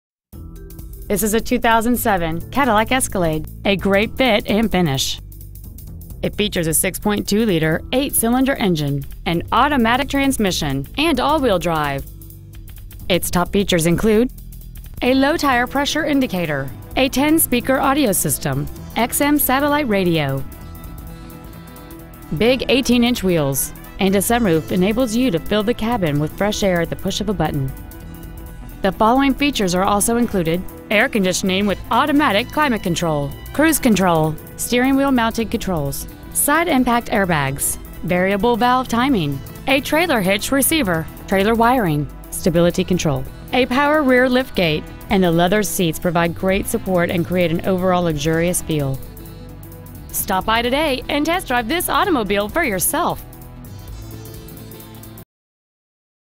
2007 Cadillac Escalade ESV Dallas TX
We are proud to present this 2007 Cadillac Escalade ESV .

 Year : 2007
 Make : Cadillac
 Model : Escalade ESV
 Engine : 6.2L Vortec 1000 VVT V8 SFI Engine
 Trans . : Automatic
 Exterior : Blue
 Miles : 147,824
 Interior : Black

 Preowned 2007 Cadillac Escalade ESV Arlington TX